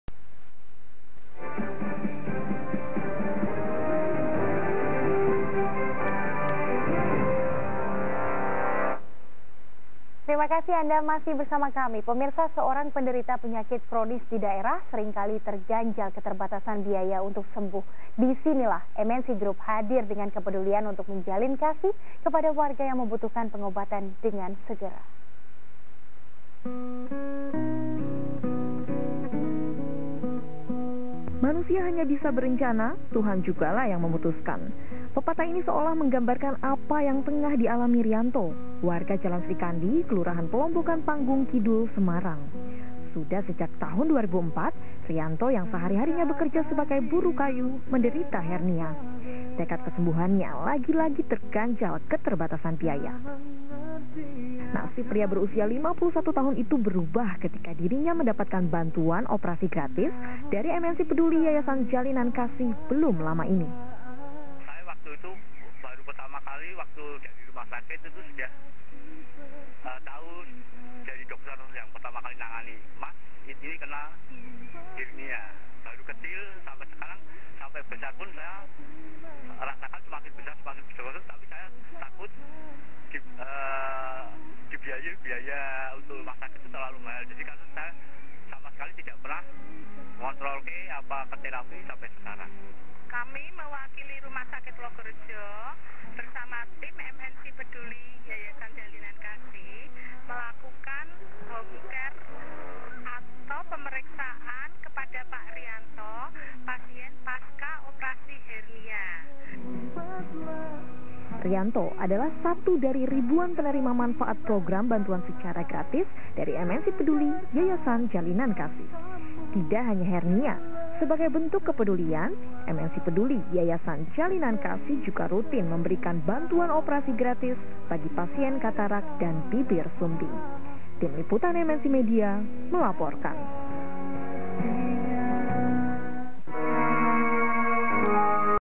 MNC Group Melalui MNC Peduli Jalinan Kasih Membantu Operasi Gratis Penderita Hernia
Seorang penderita penyakit kronis didaerah sering kali terganjal oleh keterbatasan biaya untuk sembuh disinilah MNC Group hadir dengan kepedulian  untuk menjalin kasih kepada warga yang ingin membutuh pengobatan dengan segera.